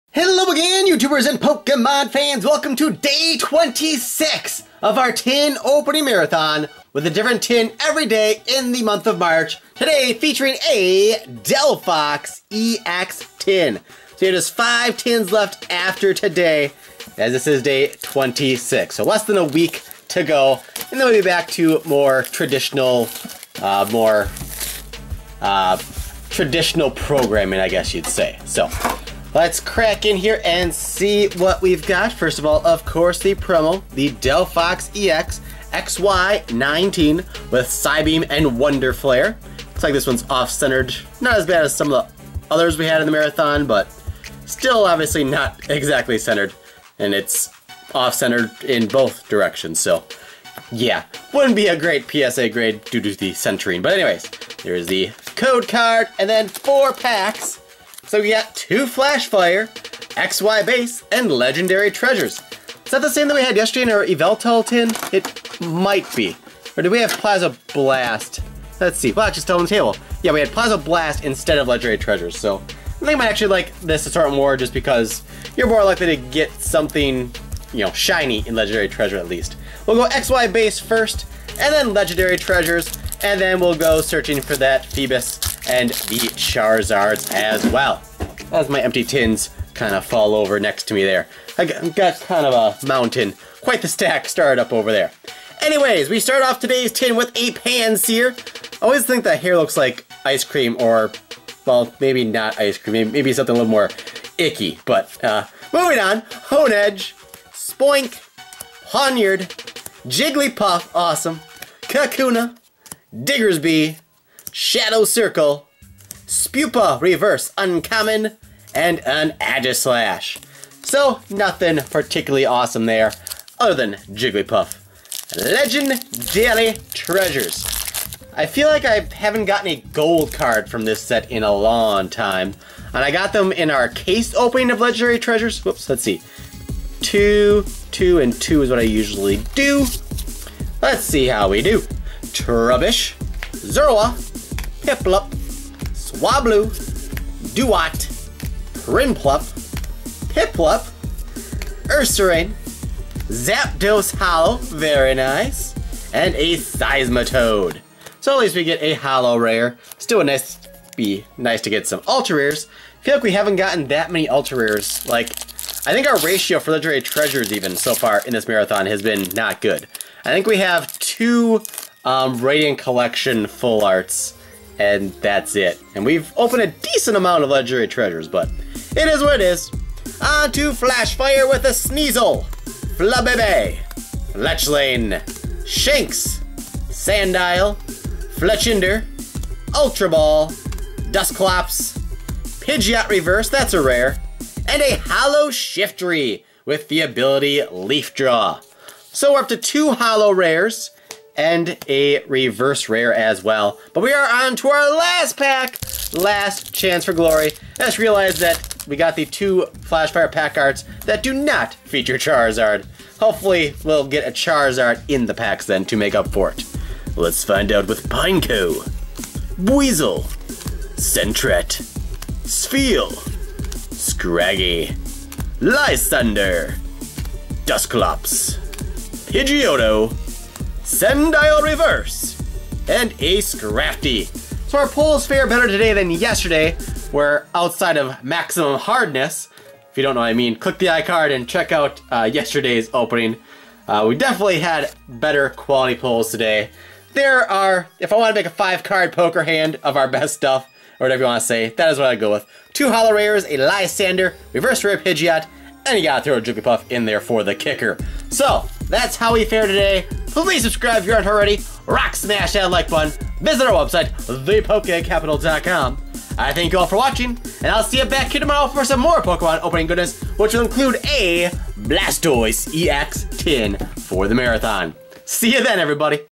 Pokemon Cards Delphox EX Tin Opening - Marathon Day 26
Pokemon Cards we're opening in today's video are from a Delphox EX Tin! The tin contains 4 booster packs and of course a Delphox EX promo card. The packs are: 2 XY Flashfire booster packs, 1 XY Base set pack, and 1 pack of Black & White Legendary Treasures.

for our livestreams!